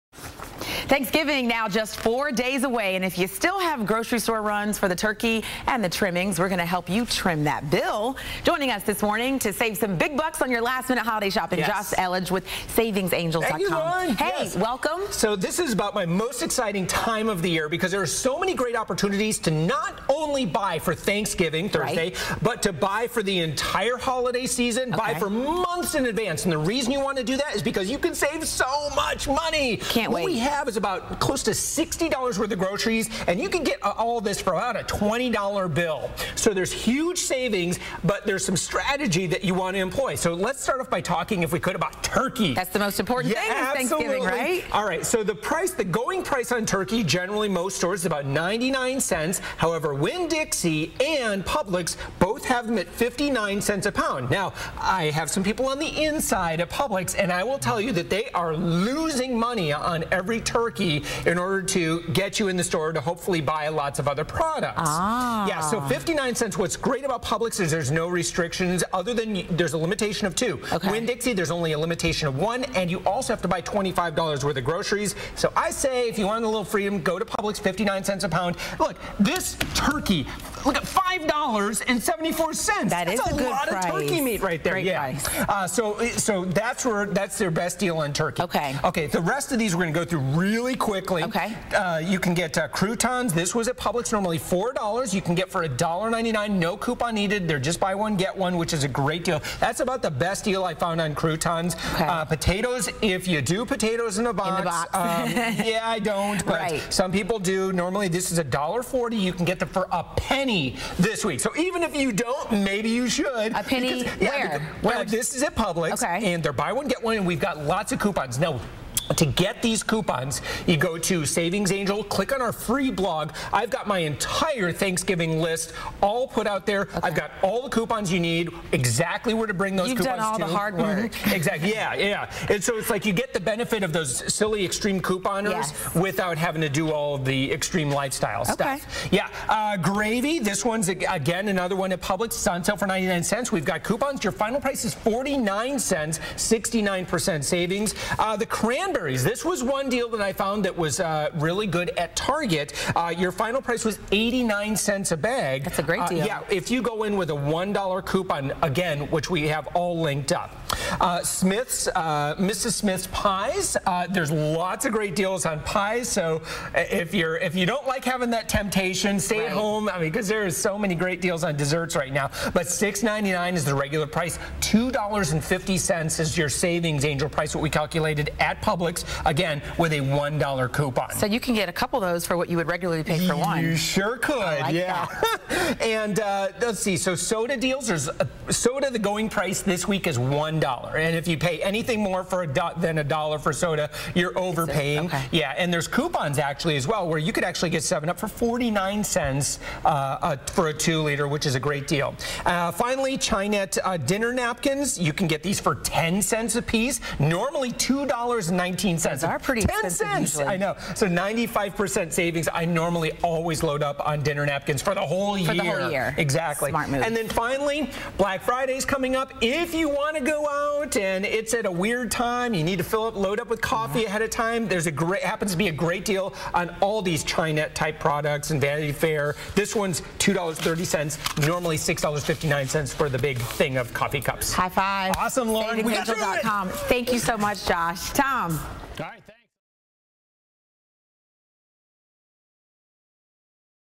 SavingsAngel.com Extreme Couponing Thanksgiving edition Fox 35 Orlando
As Seen on MyFox Orlando (WOFL Fox 35) $125 worth of groceries for about $33.28. All the details are listed here: